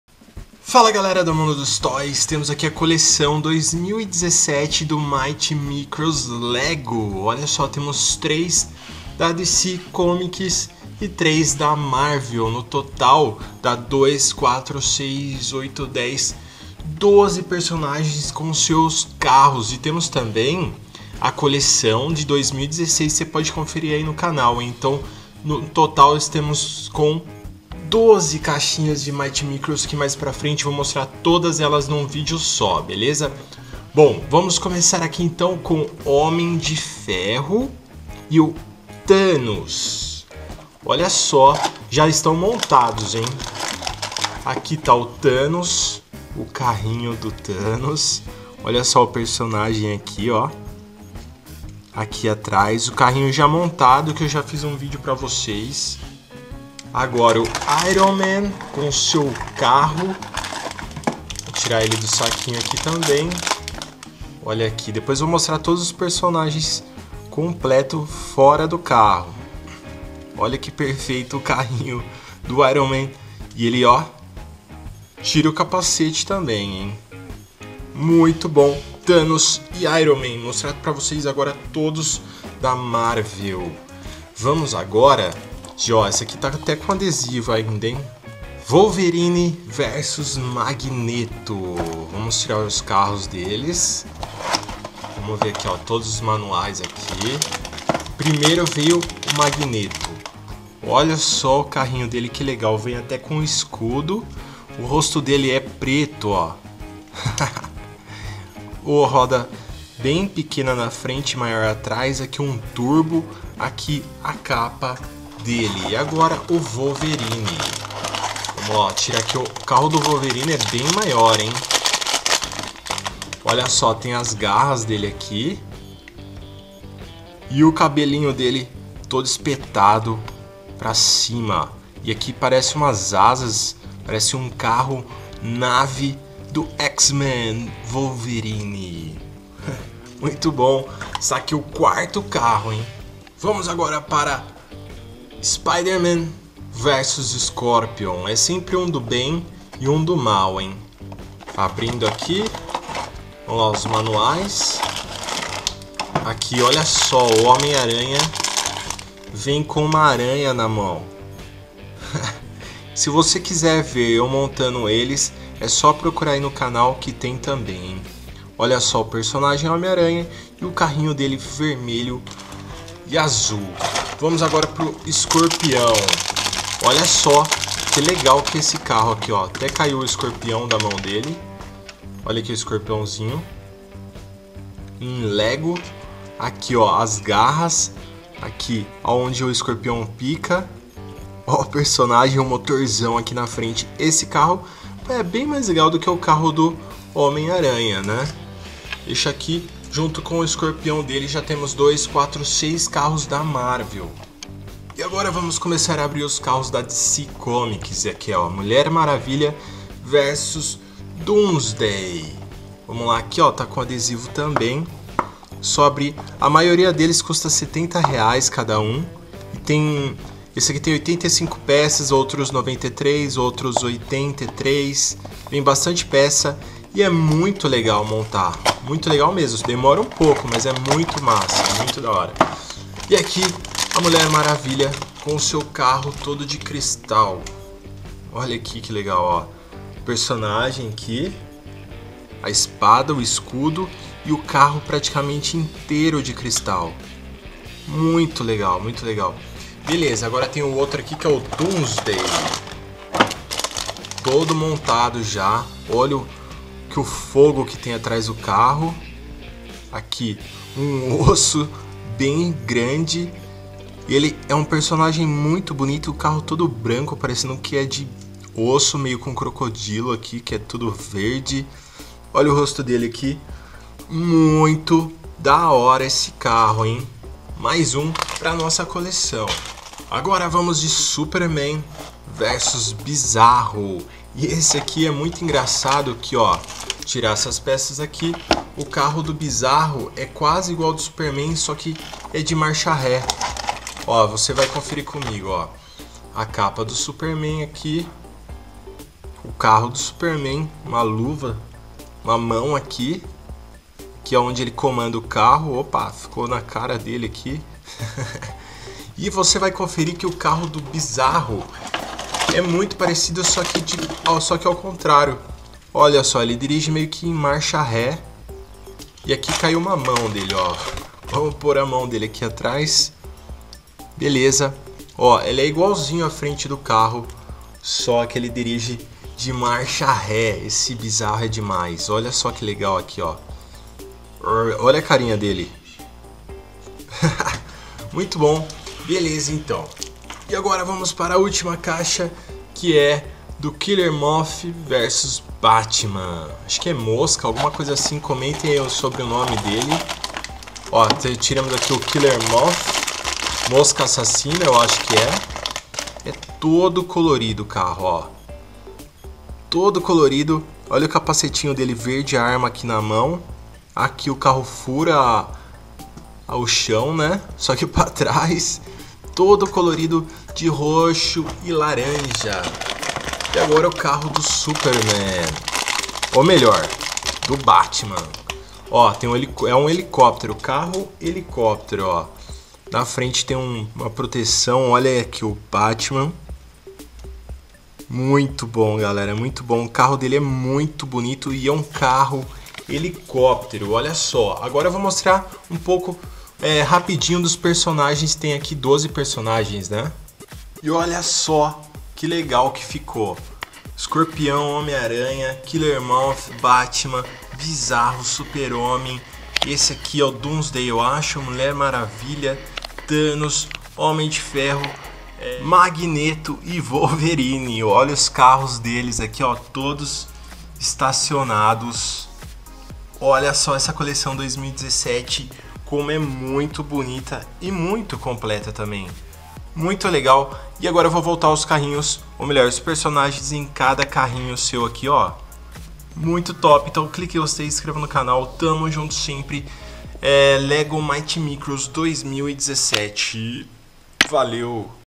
Lego Mighty Micros Coleção 2017 Super Heroes Marvel e Dc Comics
Confira os Brinquedos Lego Mighty Micros Coleção 2017 Super Heroes Marvel e Dc Comics !
No total tem 12 personagens, entre eles : Wolverine, Homem de Ferro, Batman, Mulher Maravilha, Homem Aranha e muito mais !
Cada toy custa mais ou menos 70 reais mas são muito bem feitos, Lego nunca decepciona !
Espero que tenha gostado Lego Mighty Micros Coleção 2017, confira o vídeo completo e Grande Abraço !

Mundo dos Carrinhos, Mundo das Massinhas de Brincar, Mundo Gameplay, Fritando Pneu e Mundo Roblox.